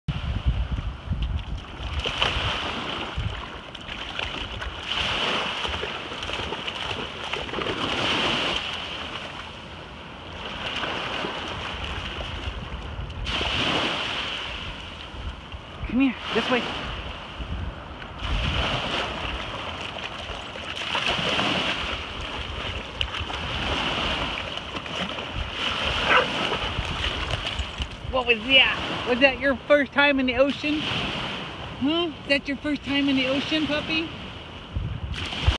Puppy's First Time Playing in the Ocean || ViralHog
Occurred on March 2021 / New Smyrna Beach, Florida, USA

"This pup was experiencing his first time in the ocean. Loved the Dog Beach at New Smyrna Inlet"